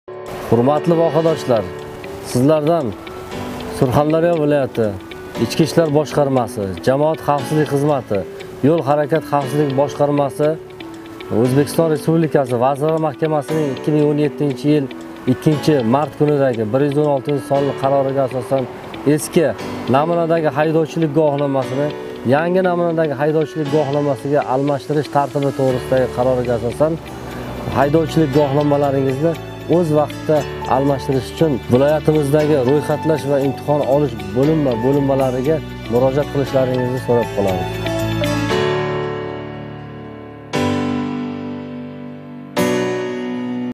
targ‘ibot Eski namunadagi guvohnomalar berilgan sanasiga muvofiq almashtiriladi:..
targ‘ibot

 Eski namunadagi guvohnomalar berilgan sanasiga muvofiq almashtiriladi:

– 2010 yilgacha bo‘lgan davrda berilganlari — 2023 yil 31 martgacha;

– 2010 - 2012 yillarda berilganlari

— 2023 yil 30 iyungacha;

– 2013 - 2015 yillarda berilganlari — 2023 yil 30 sentabrgacha;

– 2015 yildan keyin berilganlari - 2023 yil 31 dekabrgacha.